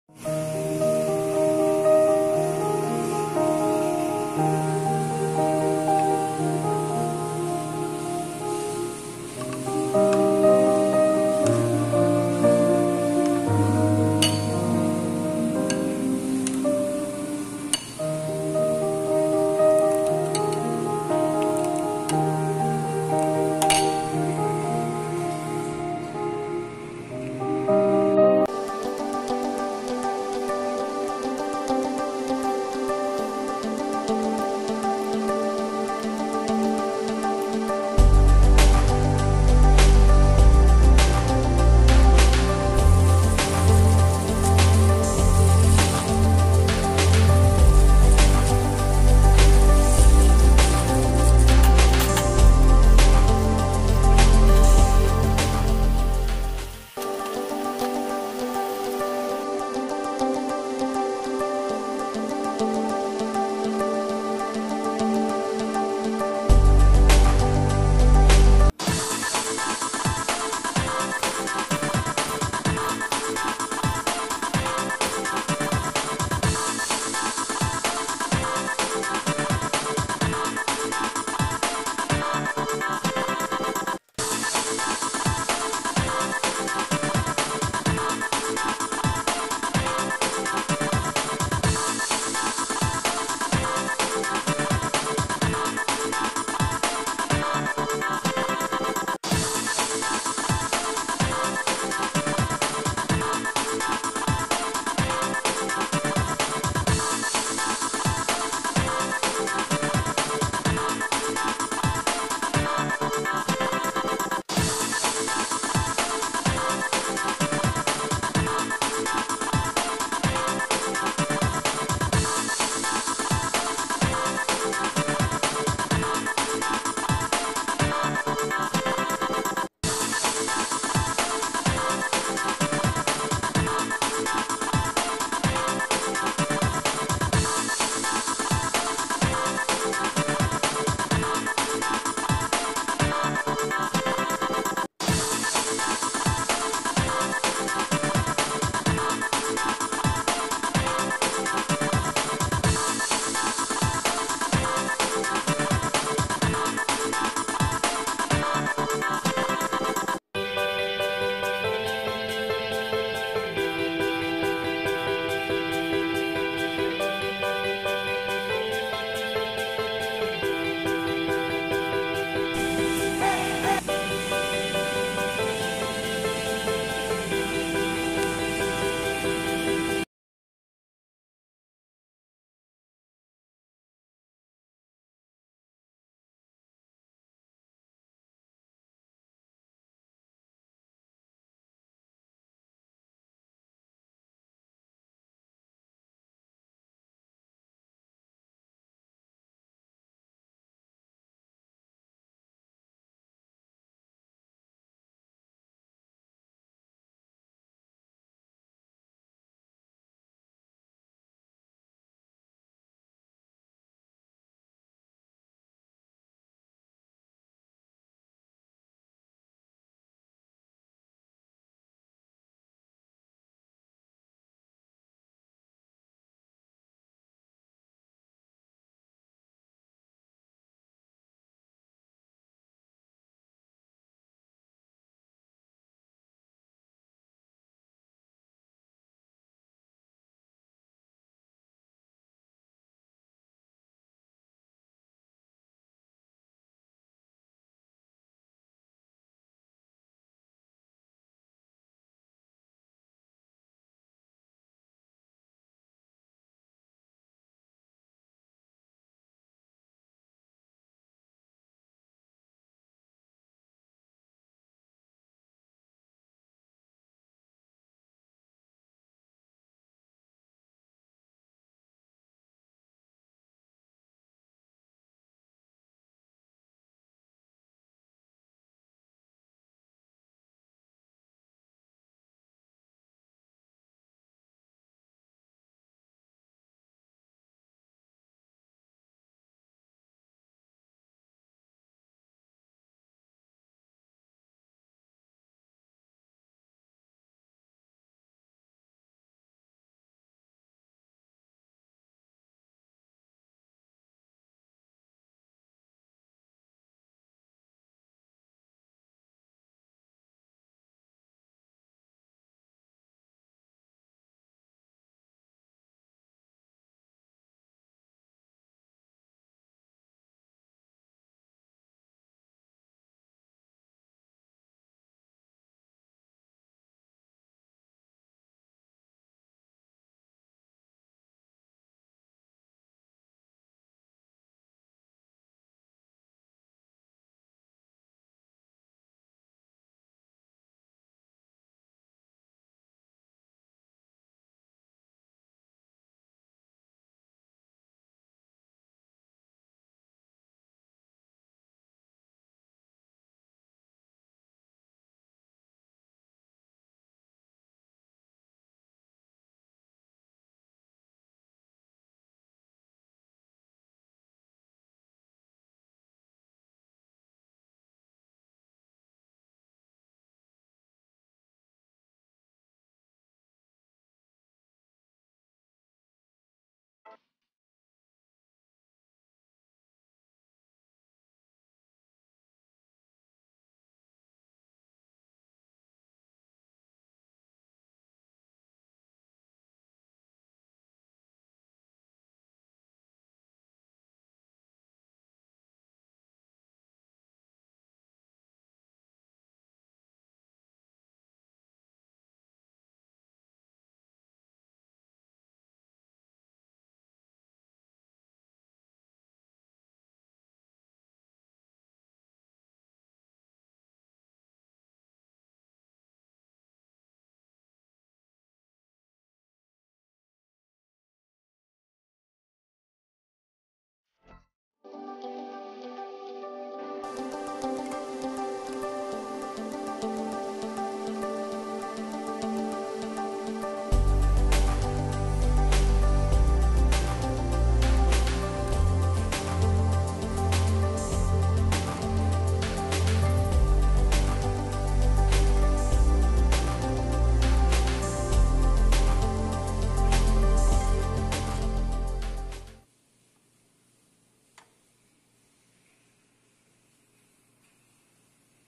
healthy green soup molokhya //मोलोकिया सुप कसरी बनाउने ??
amazing health benefits soup is make from jute or molokhya leaf ..the name is arabik molokhya..